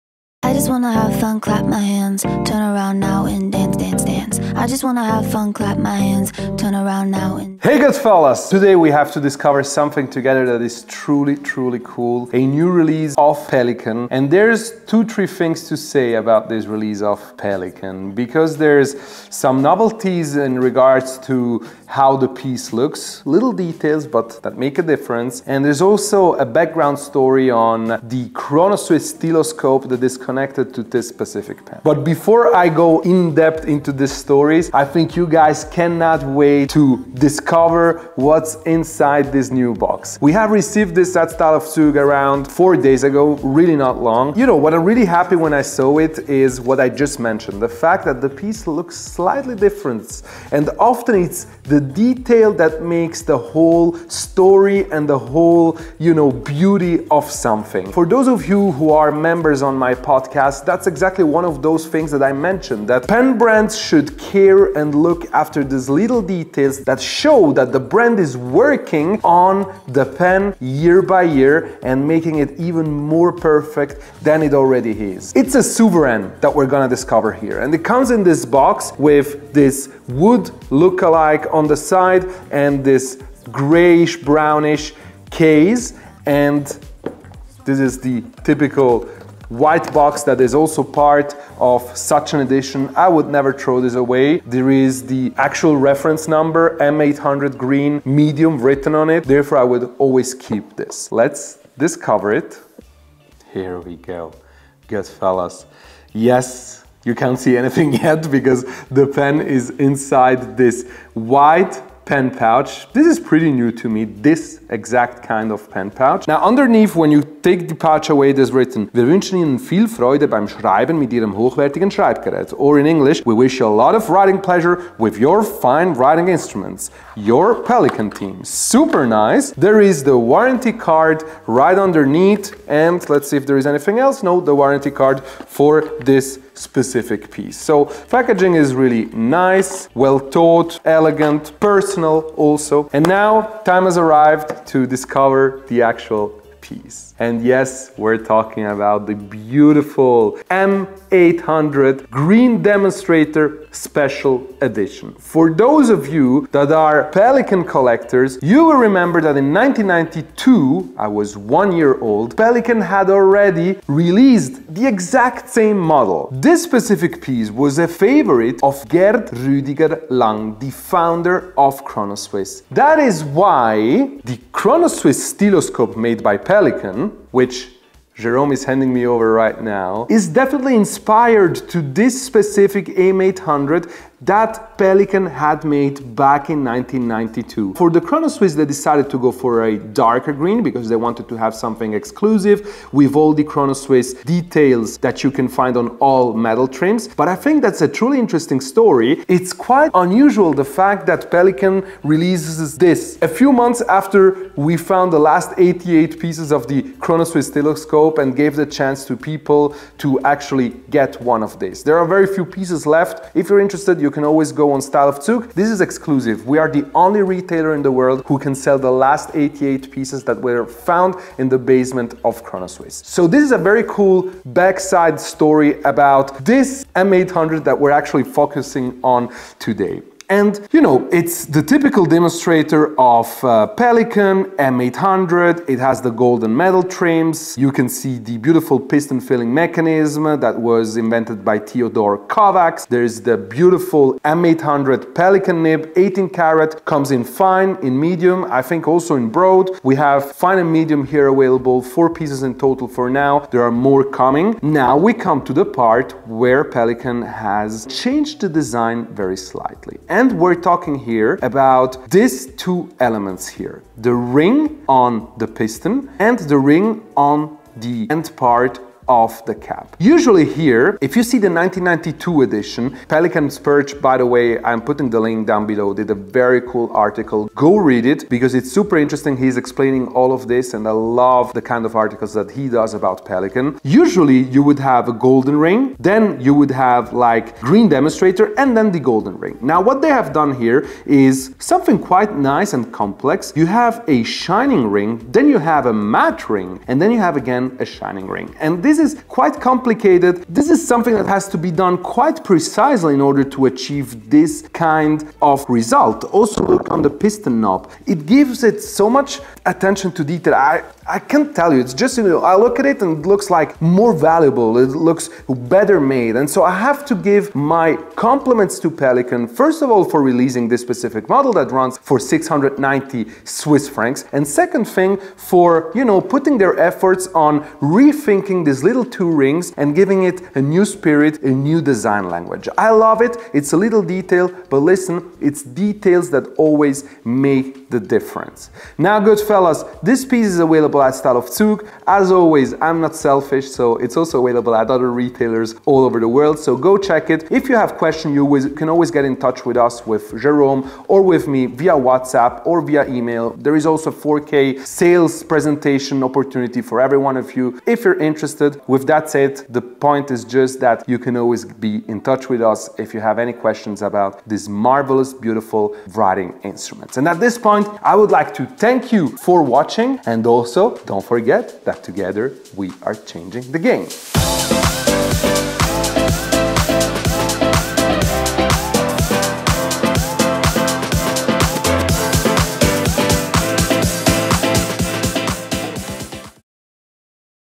PELIKAN drops a GREEN DEMONSTRATOR M800 and it's ICONIC!
Goodfellas... i think this new Green Demonstrator of Pelikan is a Must.

Follow our Style of Zug Outlet on Instagram
📱 Instagram (it's a private instagram account and we'll make sure to accept you as fast as possible)

If you want to read about the Industry's latest Novelties on inkstable.com